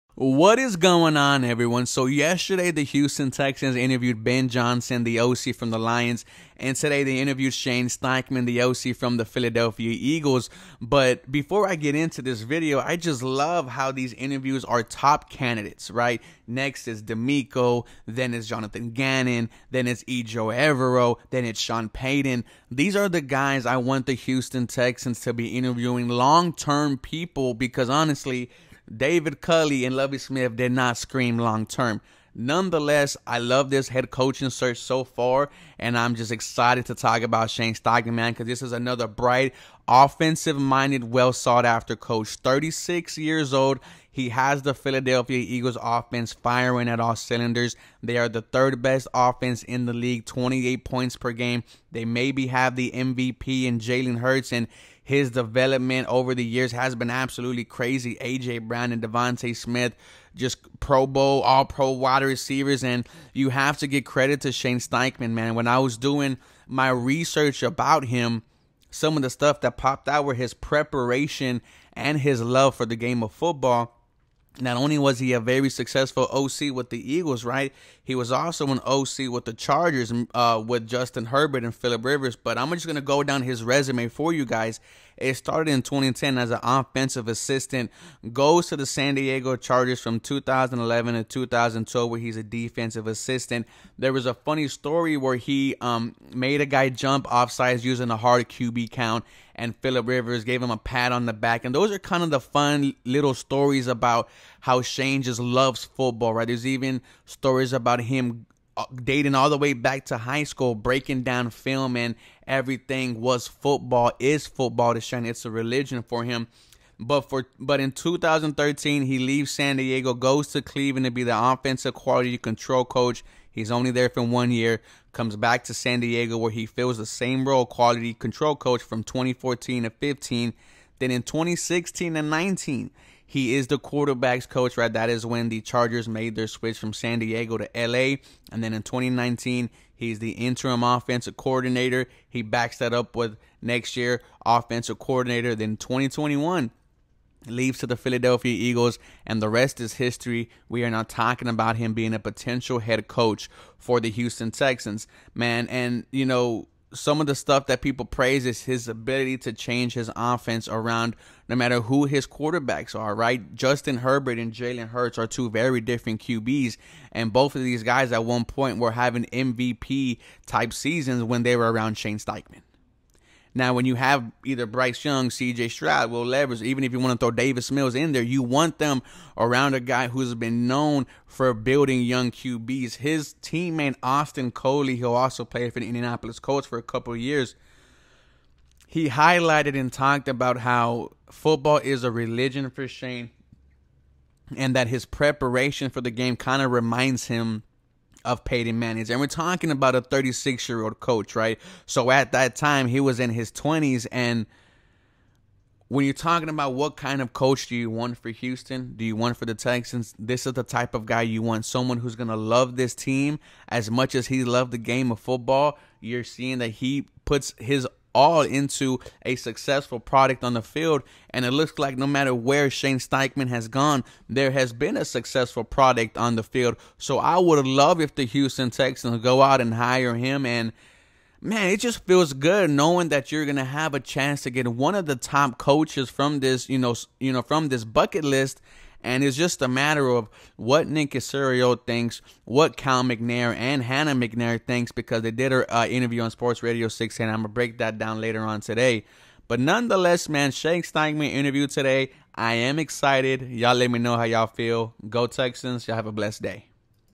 Houston Texans Interview Shane Steichen!!
The Youngest, Realist, Illist in the 713
713HSP is hosted by Mr713, a self made houstonian, who wanted to give fans a fresh perspective on our favorite team.